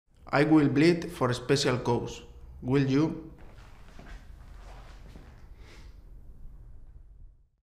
Έδωσαν το αίμα τους για τον θρόνο…. Εσύ; [Miguel Ángel Guerrero]
Οι ήρωες του Game of Thrones έδωσαν το αίμα τους για τον θρόνο….
Ο Miguel Ángel Guerrero ετοιμάζεται επίσης!
Εσύ θα δώσεις αίμα για καλό σκοπό;